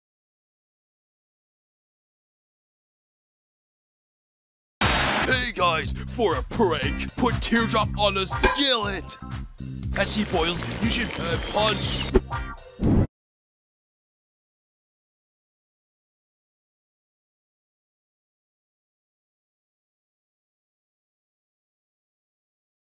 Who I vote for in BFDIA 15
You know, Pin will maybe not be eliminated, but i still dislike her because of what she did to Ruby and Book, and I adore Book!

Gift for @MediaWindowsMaker @MrMenFan677